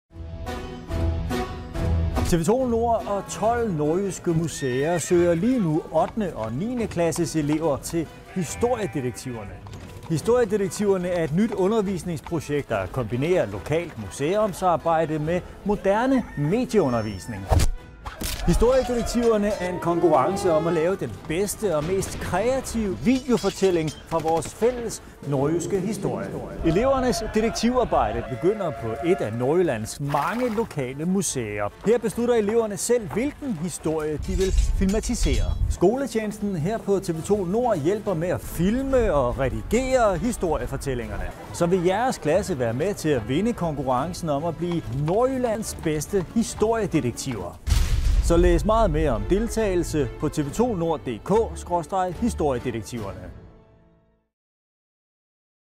TV2 Nord indleder jagten på nordjyske historiedetektiver
TV2 Nord inviterer unge nordjyder til at gå på opdagelse i den nordjyske historie i et nyt storstilet skole-museumsprojekt.